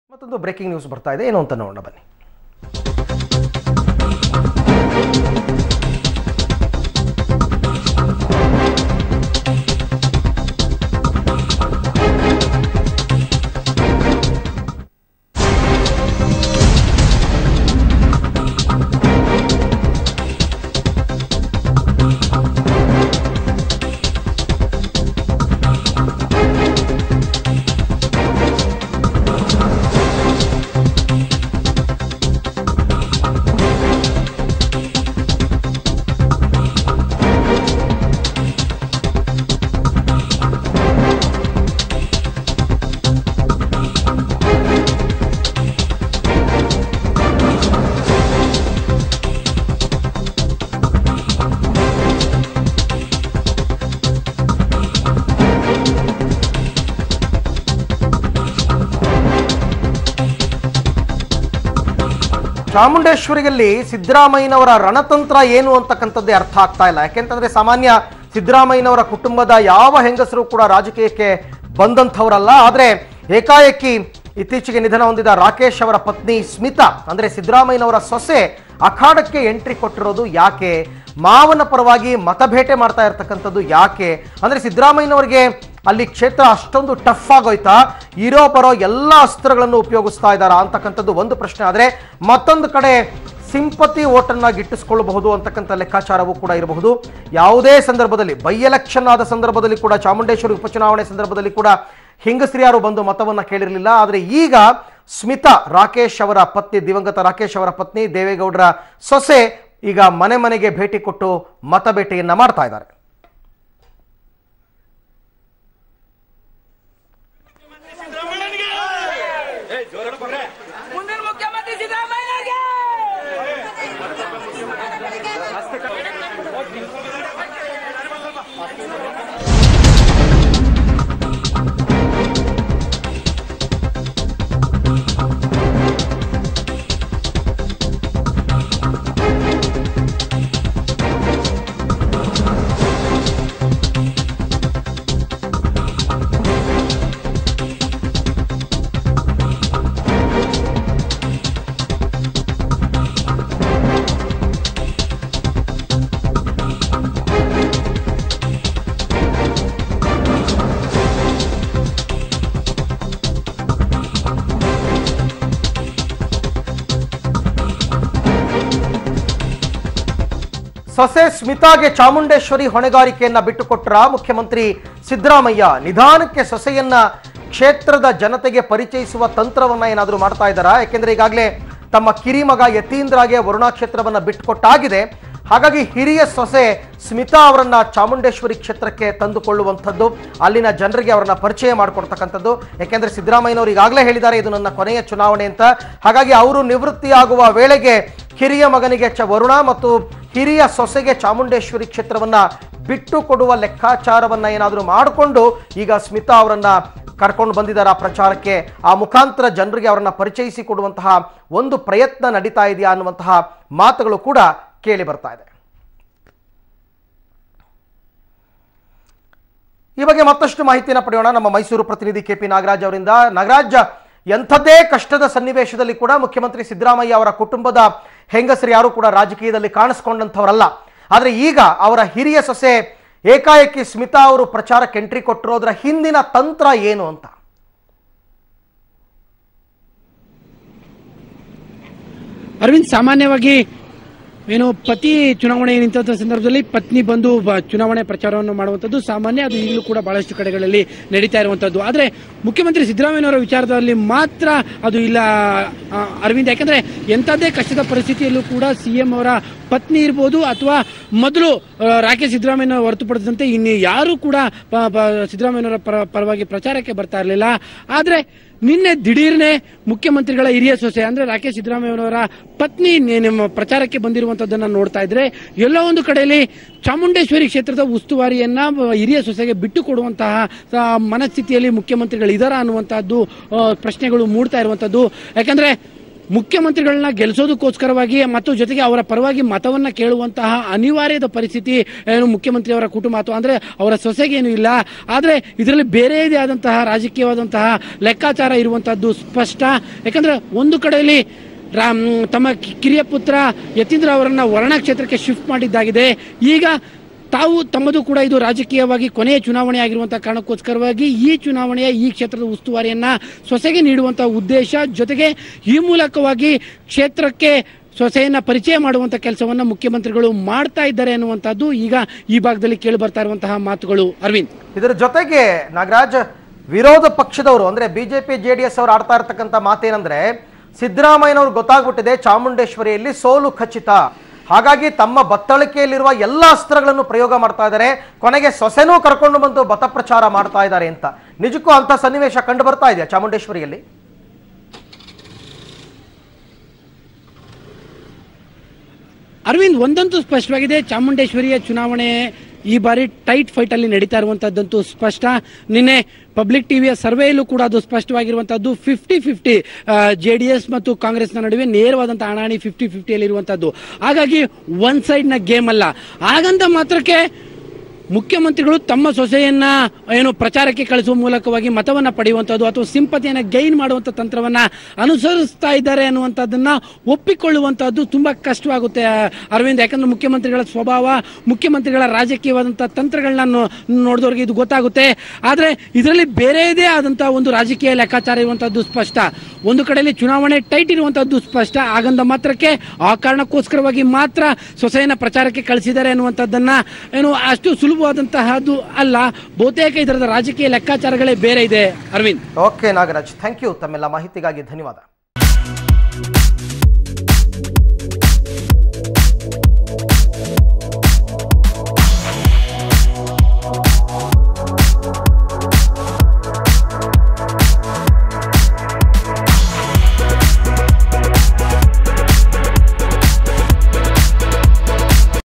What's The Reason Behind Smitha Rakesh's Entry To Election Campaign..?
What is the reason behind Smitha Rakesh Siddaarmaiah's entry to election campaigning in Chamundeshwari constituency..? Is Siddaramaiah introducing her to the people of Chamundeshwari in view of future politics or given in-charge of the constituency for the elctions..? Check out the video for more details.